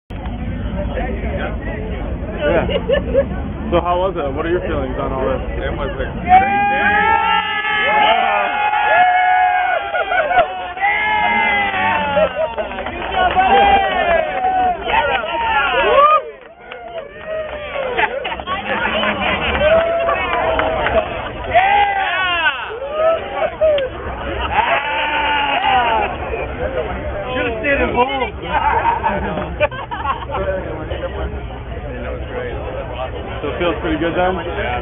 Heidi Steele Gets a Beer Shower
http Cameron and Heidi got a beer shower at the finish line in San Felipe.